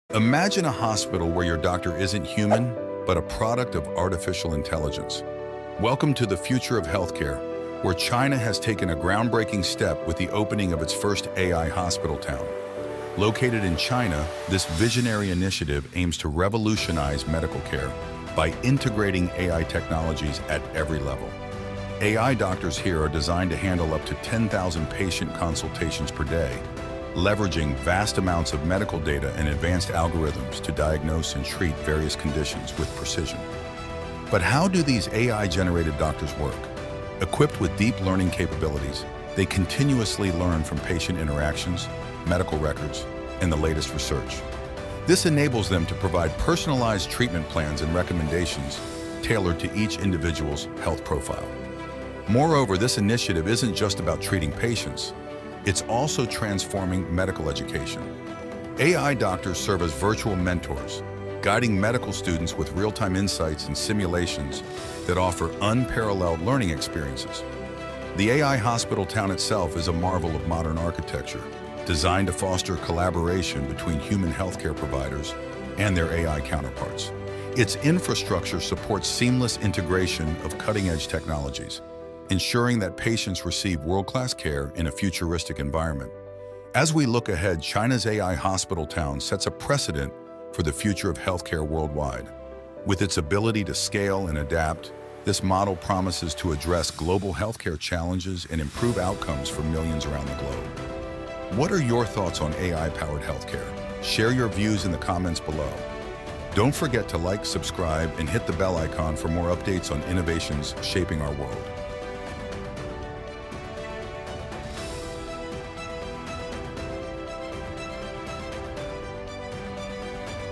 Chinas AI Hospital Revolution
Join us on an exclusive tour of China's groundbreaking AI hospital town, where artificial intelligence is transforming healthcare as we know it. From AI-generated doctors treating thousands of patients daily to revolutionary medical education advancements, discover how this futuristic initiative is setting new standards for global healthcare. Don't miss out on this peek into the future—watch now and share your thoughts on AI-powered healthcare in the comments!